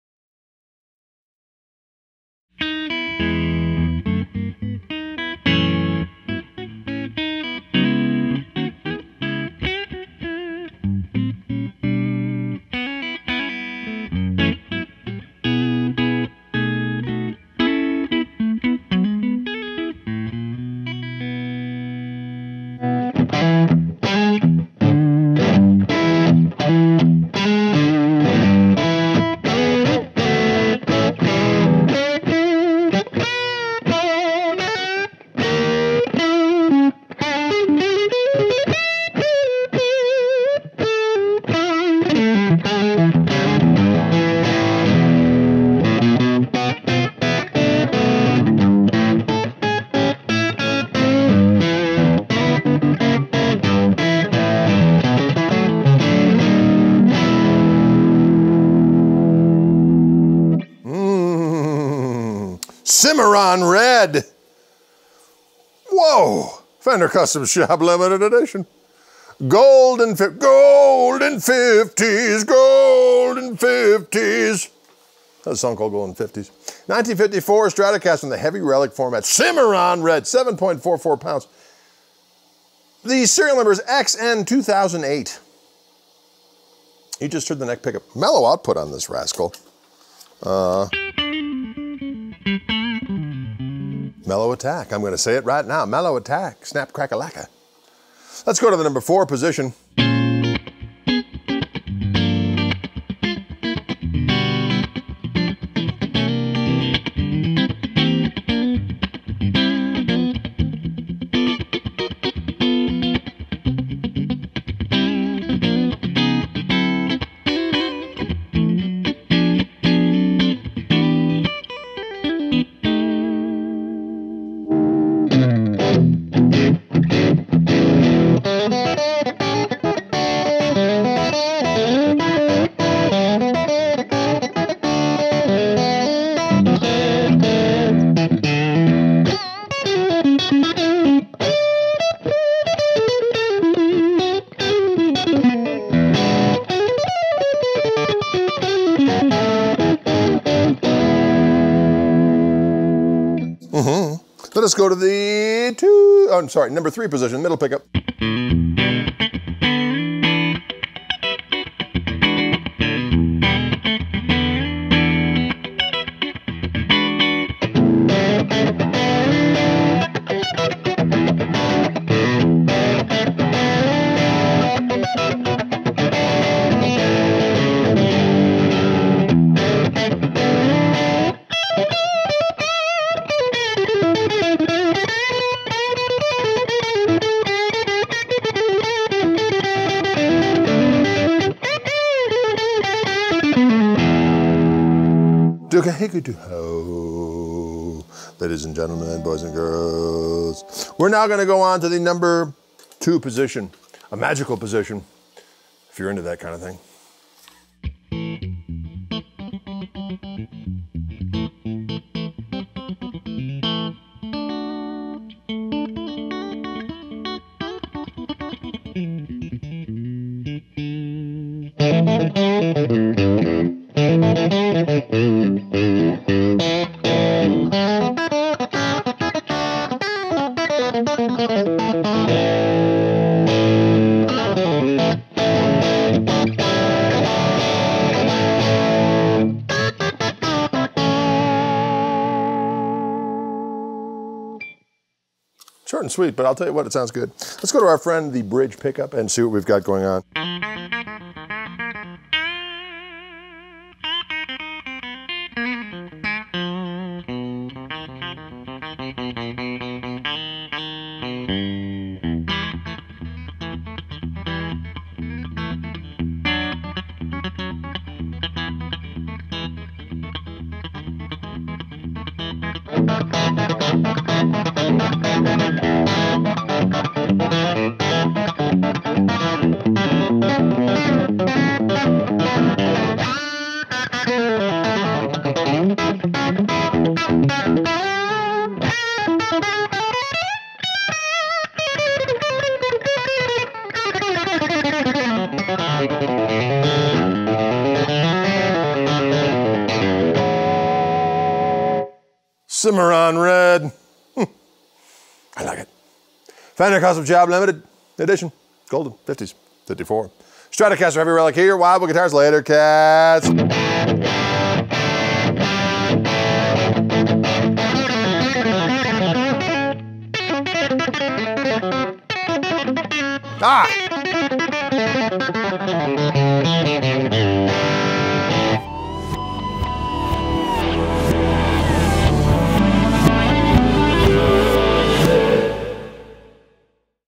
Fender Custom Shop Limited Edition Golden '50s 1954 Stratocaster Heavy Relic • SN: XN2008
The 2014 Limited Edition Golden '50s 1954 Stratocaster harkens back to a momentous period of Fender innovation. The year 1954 marks the very first production year of the Stratocaster, which profoundly influenced the exciting new playing styles of its day. To commemorate the 60th anniversary of this historic guitar, Golden '50s reimagines the allure of the original Strat for the modern player. With premium features, custom colors and a meticulous relic treatment, each one is a true masterpiece, a celebration of our first true "custom-build" instruments and a nod to a remarkable era of exuberant creativity.

Serial Number: XN2008
Brand: Fender Custom Shop
Model: Limited Edition Golden '50s 1954 Stratocaster Heavy Relic
Finish Color: Cimarron Red
Aging: Heavy Relic
Finish Type: Nitrocellulose Lacquer
Weight: 7.44 lbs
Body Wood: Ash
Neck Wood: Quarter Sawn Maple
Neck Shape: 1954 "U"
Fingerboard Radius: 9.5"
Scale Length: 25.5" (648 mm)
Width at Nut: 1.650" (42 mm)
Frets: 21 Narrow Jumbo
Pickups: 3 New 1954 Single-Coil Strat
Controls: Master Volume, 2 Tone, 5-Way Switch
Hardware : Gold
Bridge: American Vintage Synchronized Tremolo
Tuners: Vintage Style
Pickguard: Gold Anodized Aluminum
Case: Vintage Style Tweed Case
Case Candy: Strap, Cable
COA: Yes